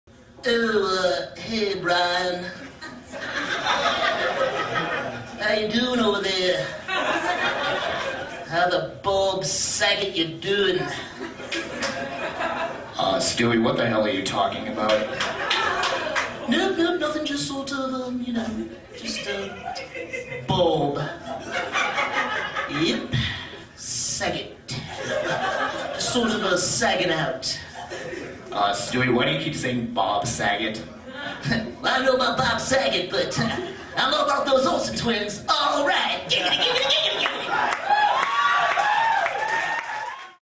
Family Tourette’s Guy (Clip)
Stand-Up from 2009 at the now defunct ARTISTS QUARTER in St. Paul, Minnesota.